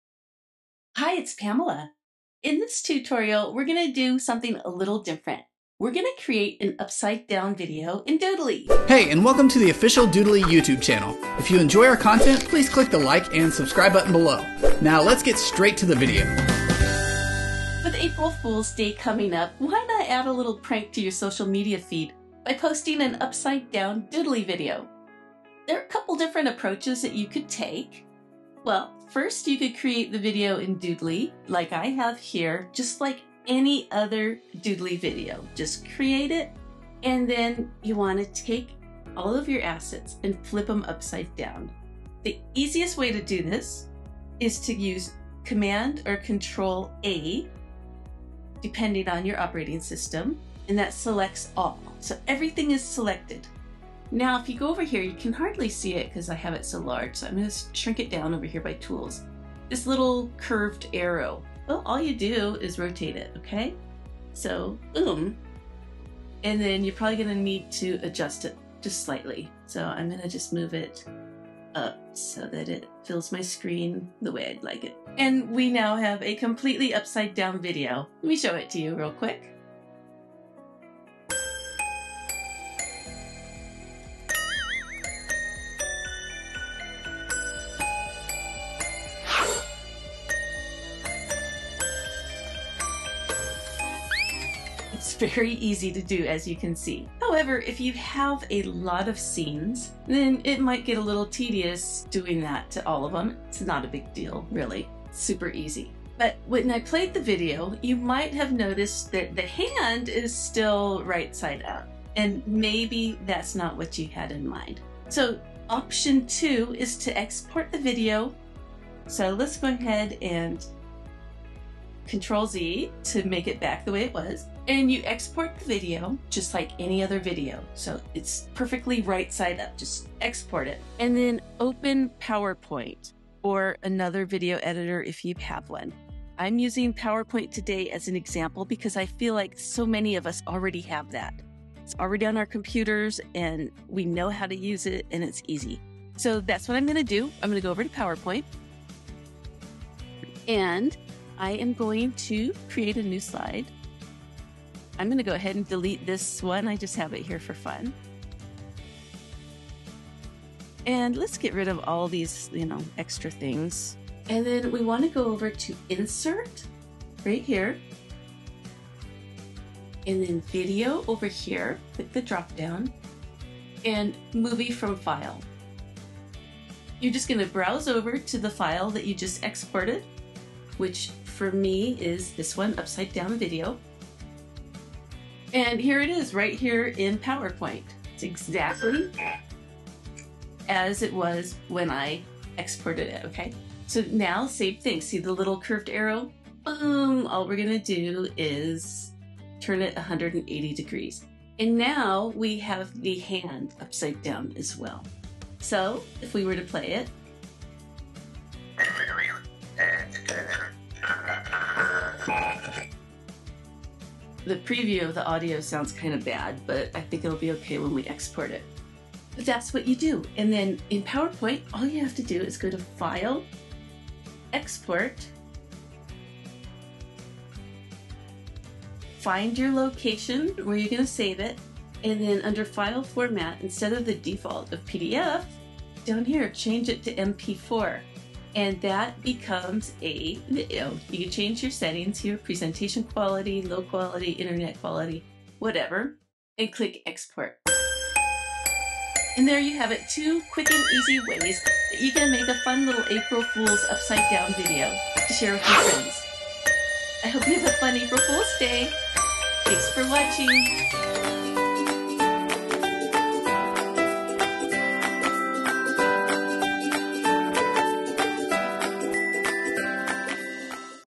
EASY April Fools' Day Video Tutorial for Doodly
In this tutorial, you’ll learn how to create an April Fool’s Day greeting in Doodly! Pamela will show you a couple different ways to make a prank video and then turn it upside down! This is a simple way to bring a smile to your friends and family's faces on social media!